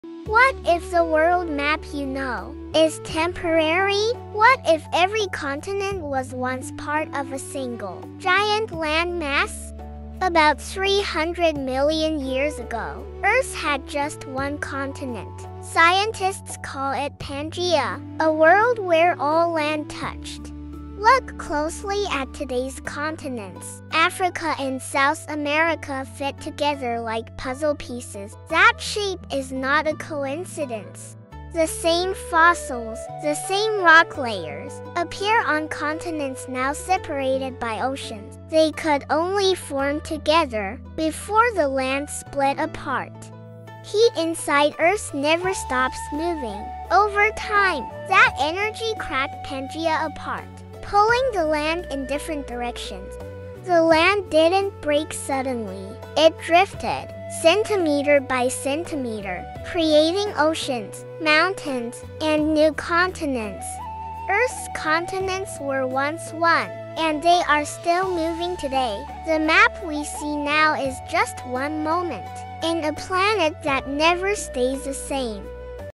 Why Was All Land Once Together?
@MorningSt8r Why was all land once together?... In this video, we explore the real science behind Pangaea, the ancient supercontinent that once connected all the land on Earth. You’ll learn how tectonic plates move, how scientists found the evidence, and why continents slowly drifted into the positions we see today. This lesson is designed to be clear, accurate, and engaging for both children and adults, helping students build a strong foundation in Earth science and geology.
What you’ll learn:
• What Pangaea was
• How continents were once connected
• Why land slowly broke apart
• How Earth’s internal energy moves plates
• Why continents are still drifting today